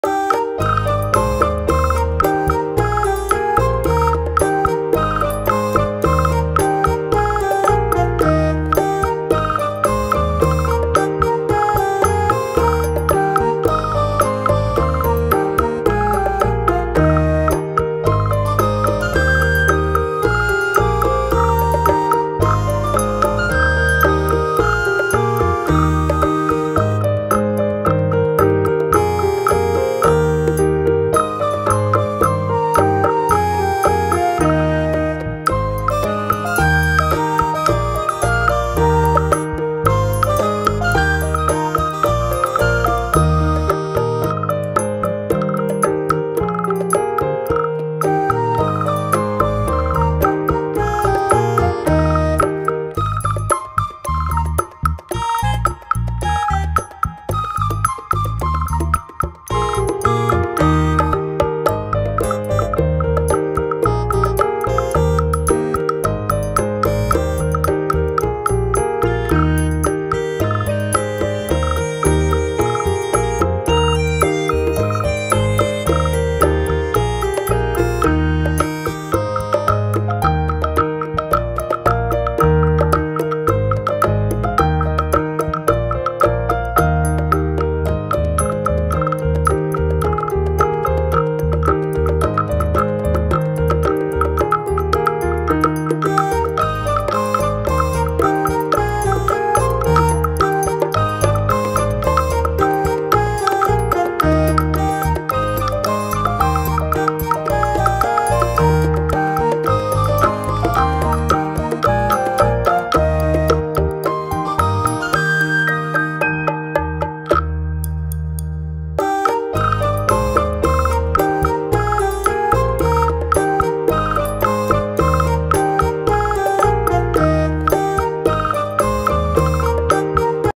Rava Upma for Baby | Baby Food Recipes |Suji Upma | Baby Food for 8+ Months
In this video we have prepared the Rava Upma for my 1 year baby so added Salt, but if you are preparing for the baby below 12 months skip the salt.

12 महीने से कम उमर कै बच्चो को चीनी और नमक ना दे